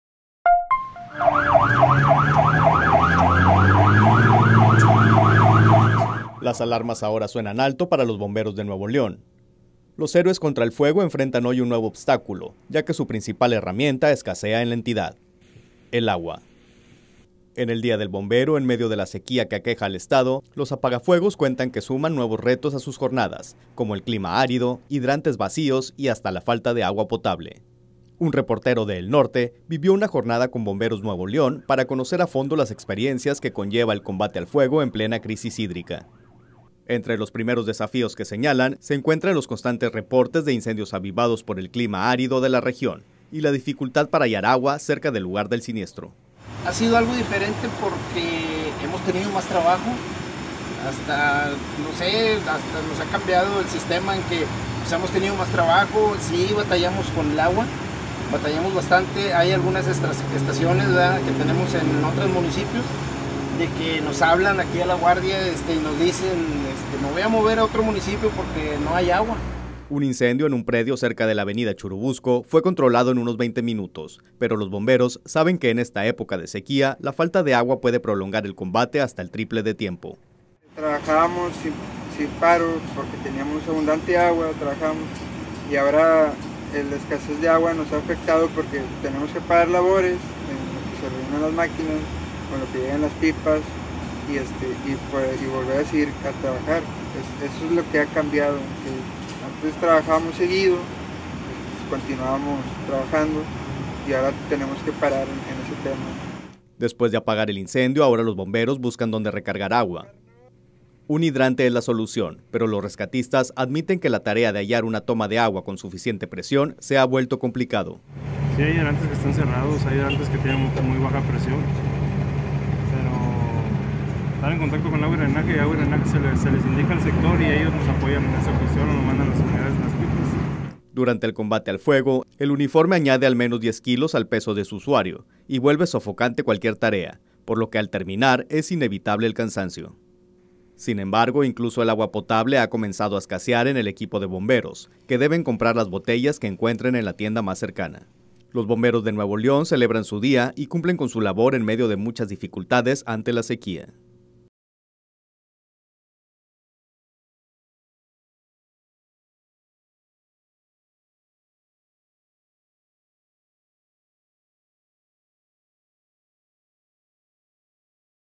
Así combaten Bomberos el fuego durante la sequía
En el DíadelBombero , los apagafuegos cuentan que suman nuevos retos a sus jornadas, como el clima árido, hidrantes vacíos y hasta la falta de agua.